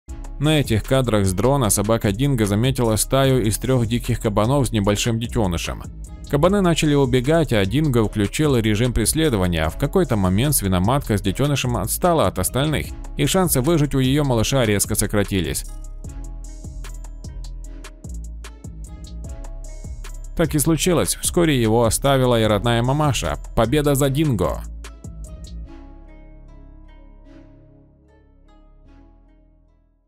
Собака Динго Против Диких Кабанов
Кабаны начали убегать, а динго включил режим преследования! В какой то момент свиноматка с детенышем отстала от остальных, и шансы выжить у ее малыша резко сократились!